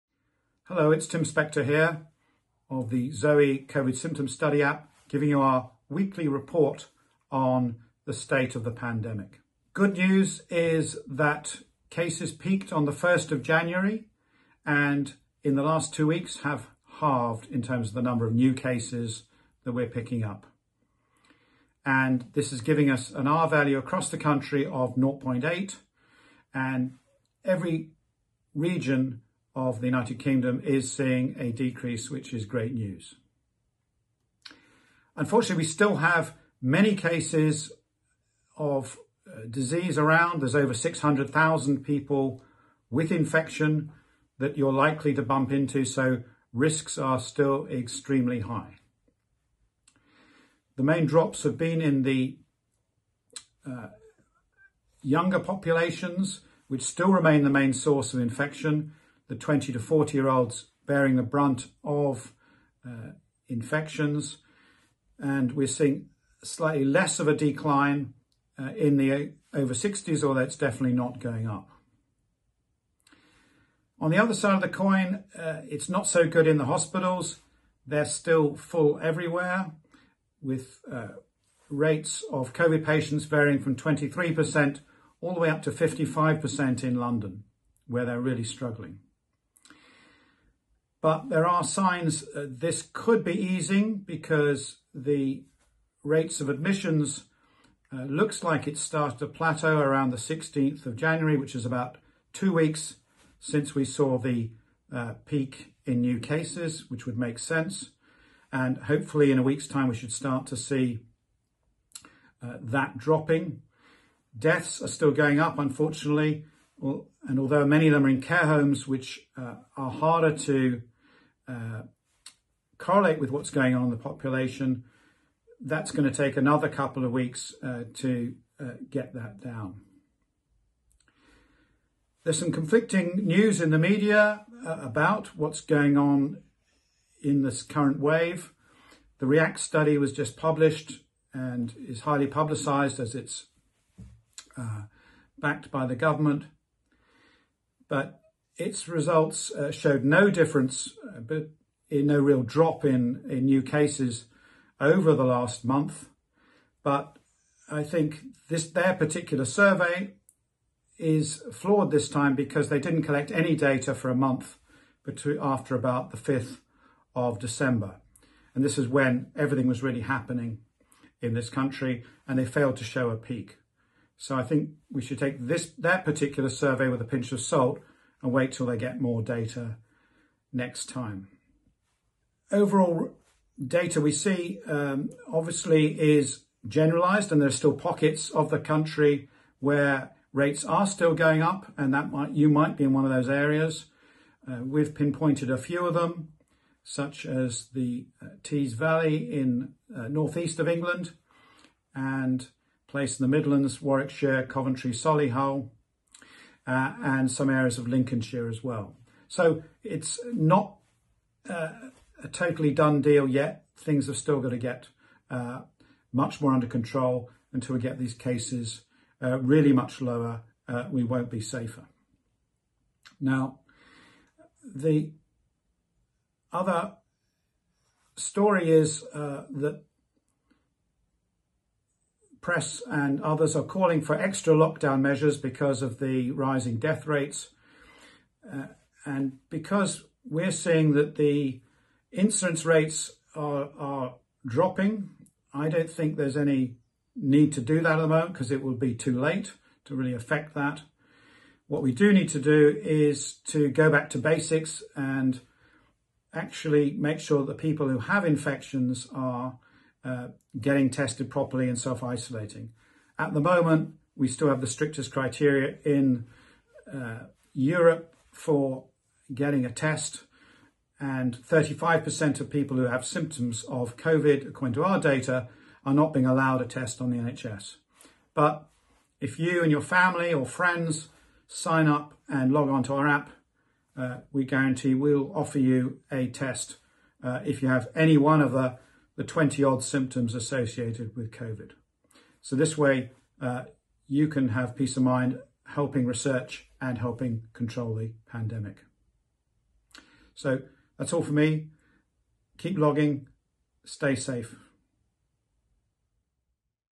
COVID rates fall to half of New Year peak
Professor Tim Spector discusses the latest ZOE COVID Symptom Study data showing rates of new cases have fallen to around half the peak seen on New Year's Day. Hospital admissions remain at an all time high, but the data gives hope that this too will begin to fall within the next two weeks with the R rate back below 1.

If you need subtitles, please click the option for subtitles at the bottom right of the video.